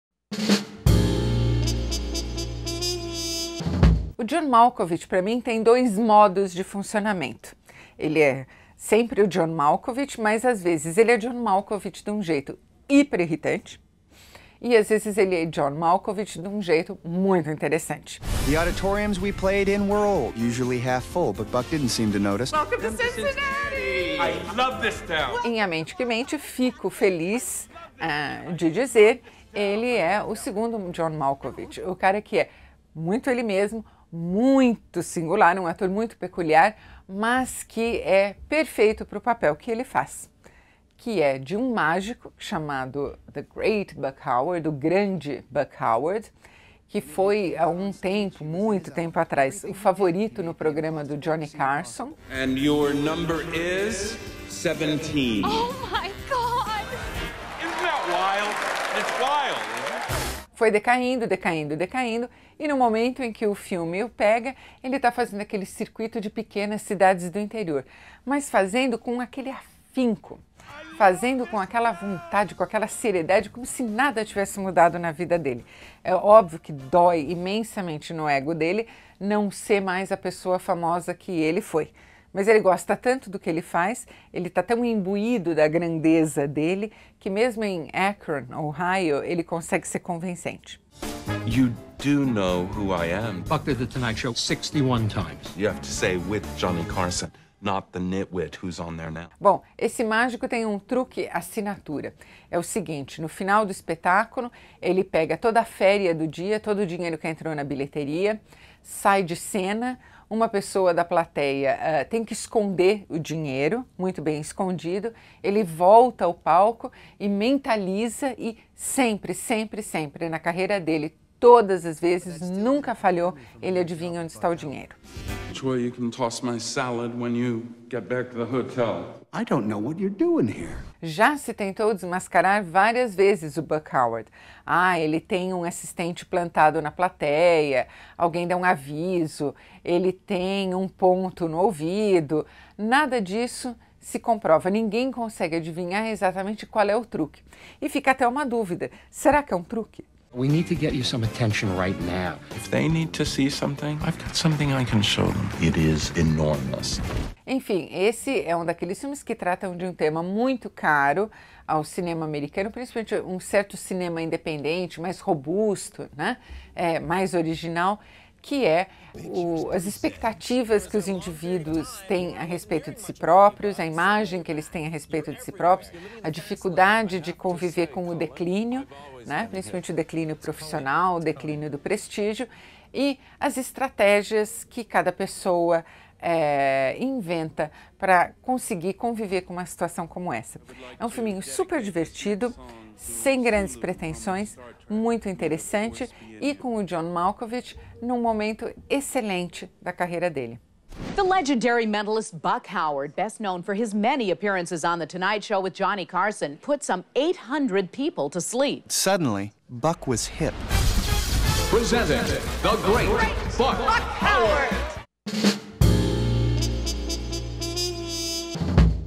A Mente que Mente
Isabela Boscov comenta a brilhante atuação de John Malkovich como um mágico decadente em 'A Mente que Mente'. Com: Colin Hanks, Tom Hanks e Emily Blunt.